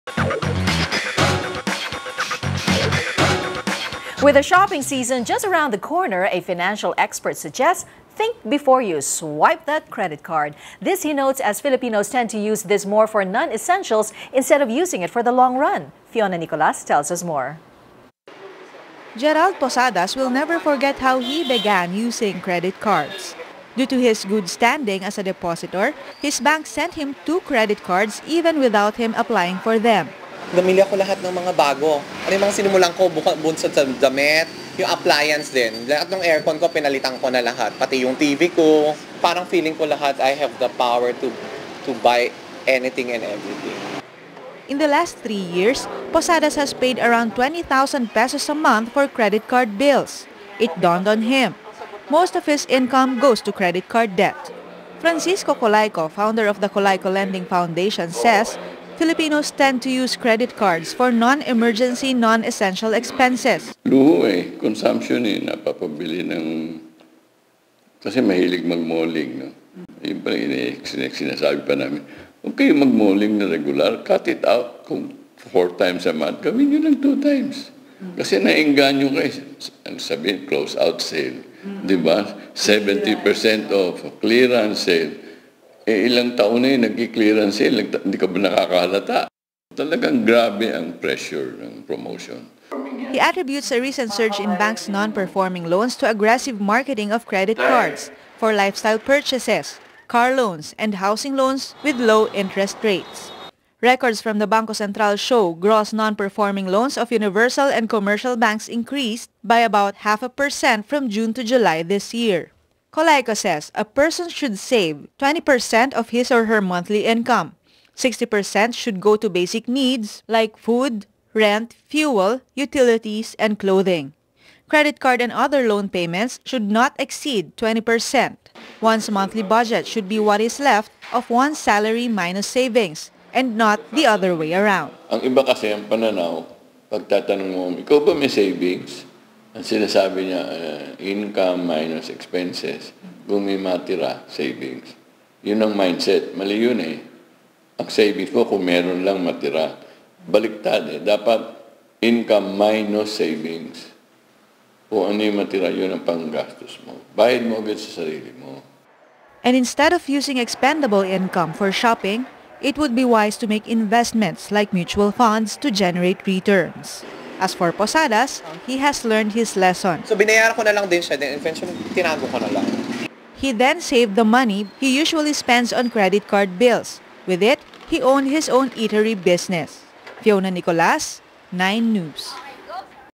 The Good And Bad With Credit Cards
With the shopping season just around the corner a financial expert suggests: think before you swipe that credit card.This he notes, as Filipinos tend to use this more for non-essentials instead of using it for the long run. Fiona Nicolas tells us more.